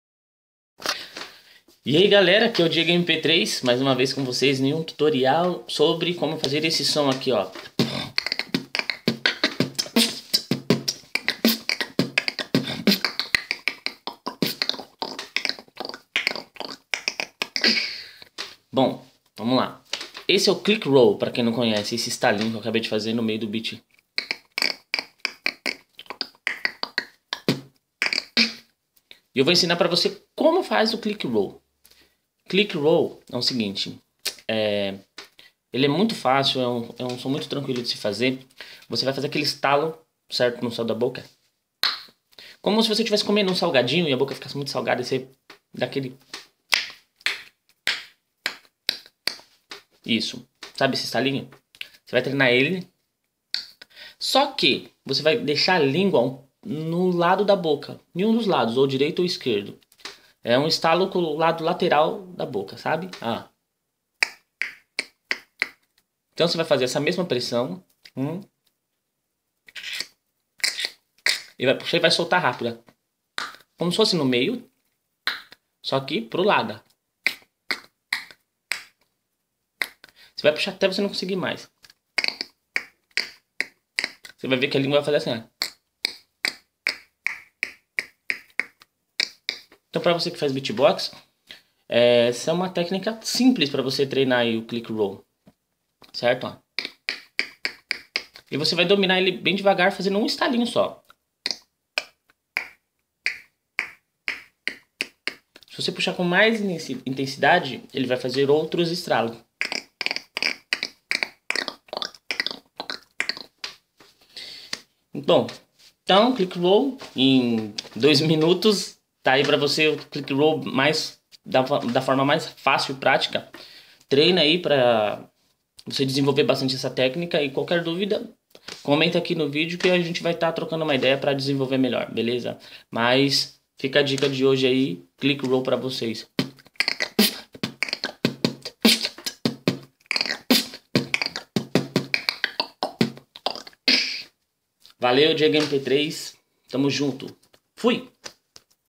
Tutorial Beatbox #1 Click Roll ‪
Técnica básica de como aprender o Click Roll.